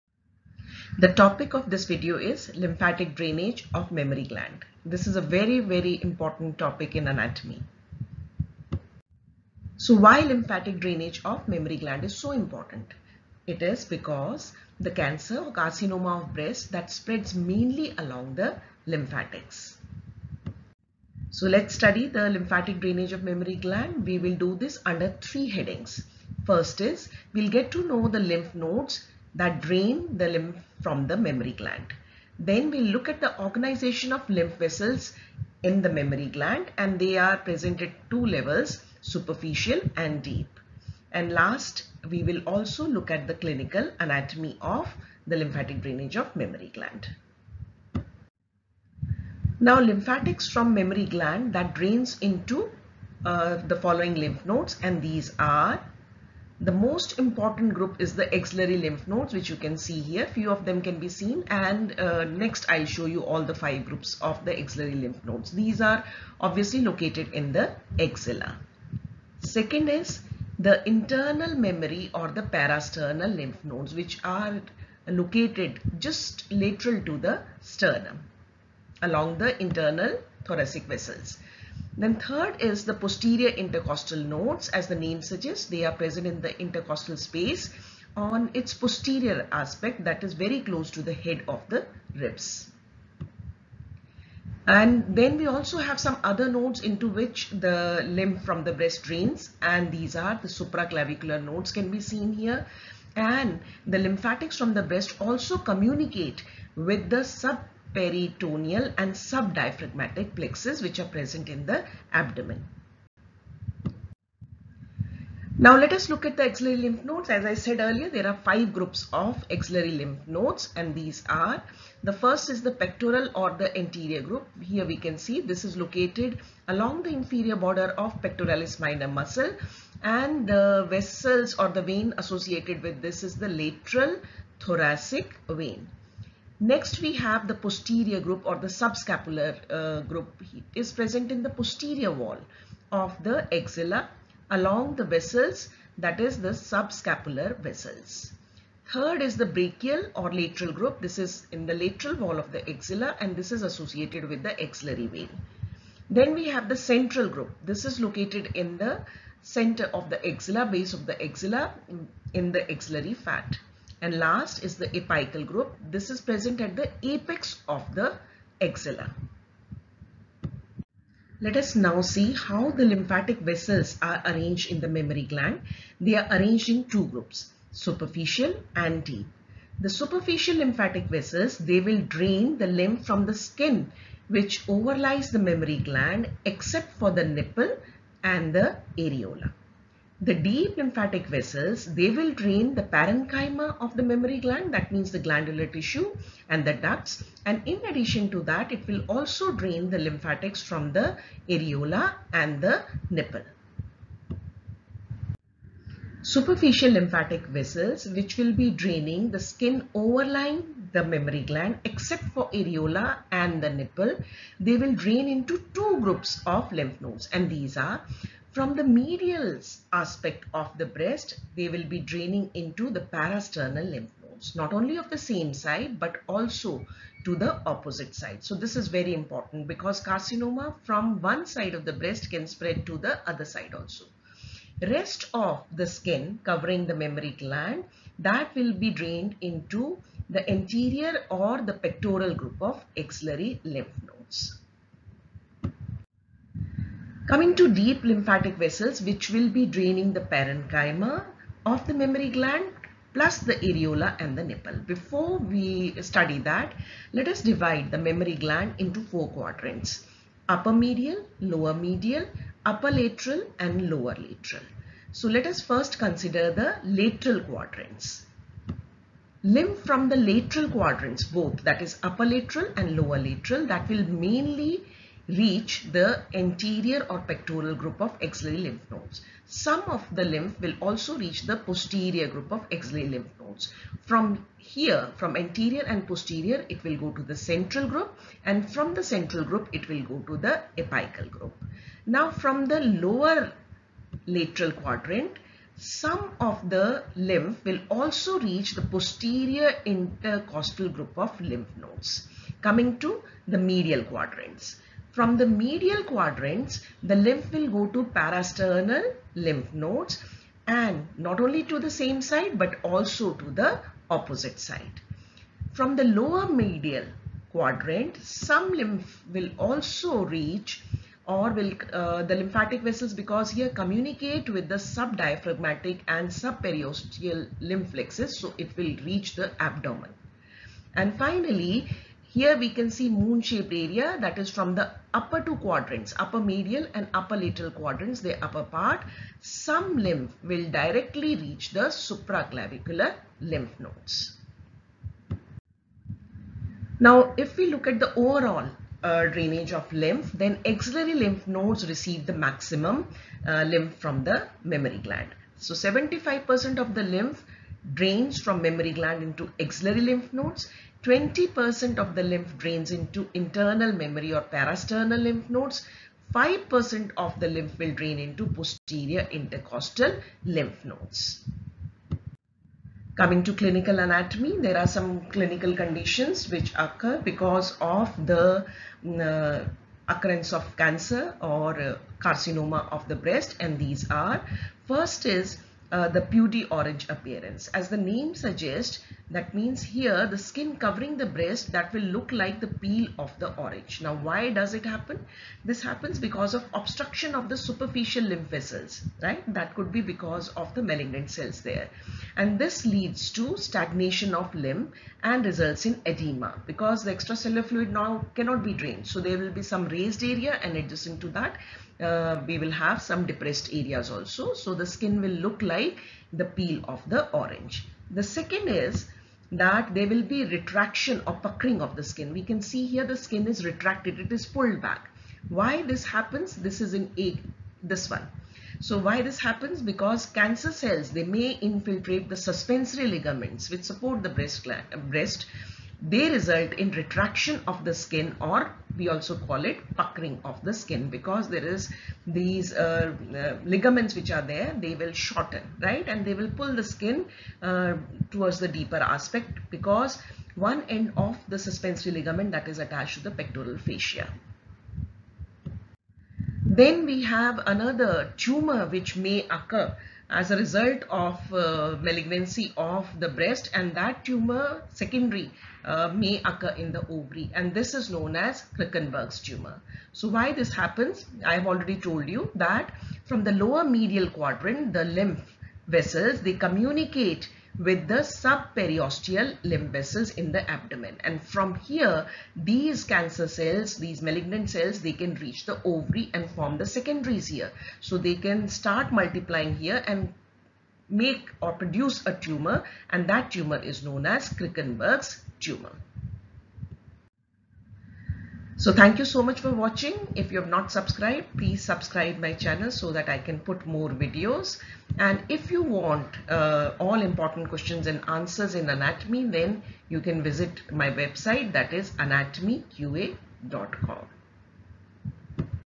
Lymphatic Drainage of Breast| Mammary Gland | Anatomy Important Topic
This is to simplify and make easy to understand the lymphatic drainage of mammary gland.
What does Peau d'orange indicate?
This is about lymphatic drainage of breast or mammary gland.
Video Highlights:
Simple explanation.
Easy to draw diagrams.
Why lymphatic drainage of breast is important?
Which lymph nodes drain the breast?
How ar lymph vessels arranged in breast?
What is Peau -de orange of breast?
What causes Krukenberg tumor?
Where does a Krukenberg tumor originate?

Links of other videos on upper limb